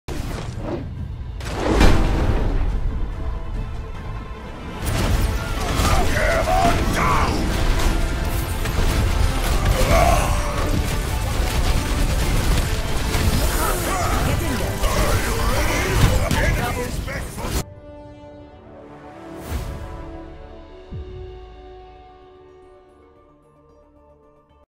Swagging On Trash Players
This was my last game of the day. I had won 4 games in the span of 40 minutes.